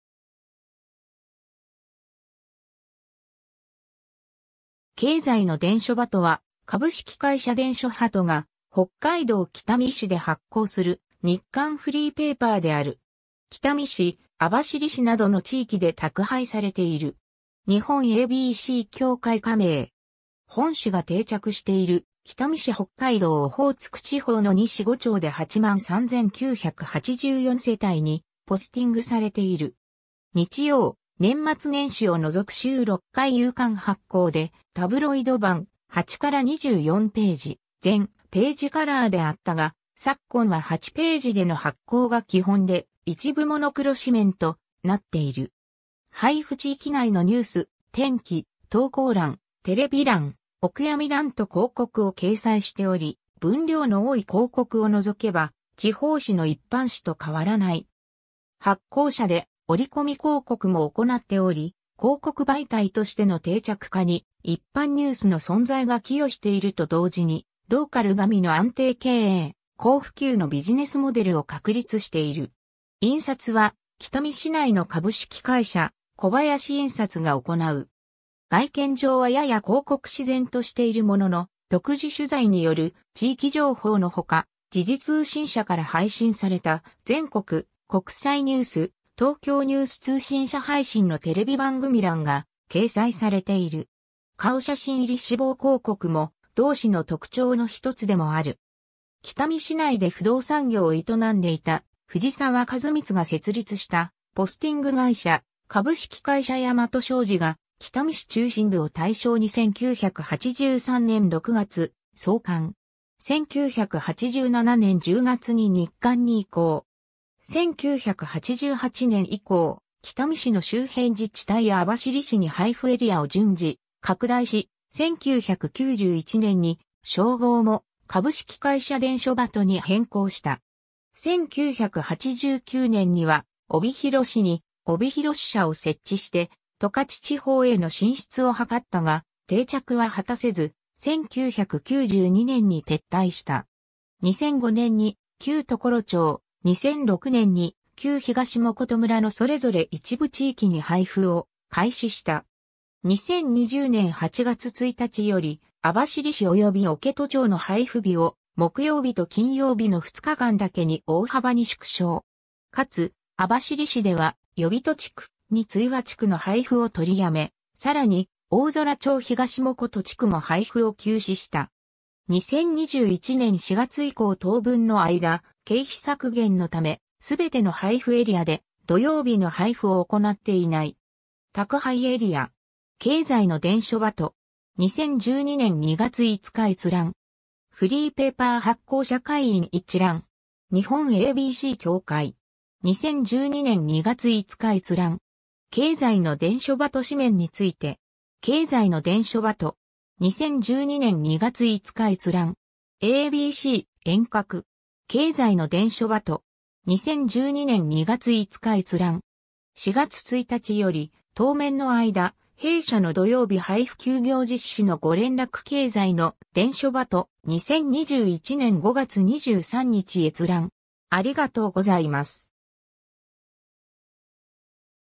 経済の伝書鳩
経済の伝書鳩（けいざいのでんしょばと、通称「鳩」）は、株式会社伝書鳩が、北海道北見市で発行する日刊フリーペーパーである。
北見市・網走市などの地域で宅配されている。
日本ABC協会加盟。
 本紙が定着している北見市 北海道オホーツク地方の2市5町（北見市、網走市、訓子府町、置戸町、津別町、美幌町、大空町）で8万3,984世帯（2020年8月実数）にポスティング（戸別宅配）されている。
日曜・年末年始を除く週6回夕刊発行で、タブロイド判、8 - 24ページ、全ページカラーであったが、昨今は8ページでの発行が基本で、一部モノクロ紙面となっている。
 配布地域内のニュース、天気、投稿欄、テレビ欄、お悔やみ欄と広告を掲載しており、分量の多い広告を除けば地方紙の一般紙と変わらない。
発行社で折り込み広告も行っており、広告媒体としての定着化に一般ニュースの存在が寄与していると同時に、ローカル紙の安定経営・高普及のビジネスモデルを確立している。
印刷は北見市内の株式会社小林印刷が行う。
 外見上はやや広告紙然としているものの、独自取材による地域情報の他、時事通信社から配信された全国・国際ニュース、東京ニュース通信社配信のテレビ番組欄が掲載されている。
顔写真入り死亡広告も、同紙の特徴の一つでもある。
 北見市内で不動産業を営んでいた藤澤和光が設立したポスティング会社、株式会社大和商事が、北見市中心部を対象に1983年6月創刊。
1987年10月に日刊に移行。
1988年以降、北見市の周辺自治体や網走市に配布エリアを順次拡大し、1991年に商号も「株式会社伝書鳩」に変更した。
 1989年には帯広市に帯広支社を設置して十勝地方への進出を図ったが定着は果たせず、1992年に撤退した。
 2005年に旧常呂町、2006年に旧東藻琴村のそれぞれ一部地域に配布を開始した。
 2020年8月1日より、網走市(14,704枚)および置戸町(1,069枚)の配布日を、木曜日と金曜日の2日間だけに大幅に縮小。
かつ、網走市では呼人地区・二ツ岩地区の配布を取りやめ、更に、大空町東藻琴地区も配布を休止した。
 2021年4月以降当分の間、経費削減のため、すべての配布エリアで土曜日の配布を行なっていない。
 ^ “宅配エリア”. 経済の伝書鳩. 2012年2月5日閲覧。
 ^ “フリーペーパー発行社会員一覧（地域区分）”. 日本ABC協会. 2012年2月5日閲覧。
 ^ “経済の伝書鳩紙面について”. 経済の伝書鳩. 2012年2月5日閲覧。
 ^ a b c “沿革”. 経済の伝書鳩. 2012年2月5日閲覧。
 ^ ４月１日より当面の間、弊社の土曜日配布休業実施のご連絡 経済の伝書鳩、2021年5月23日閲覧。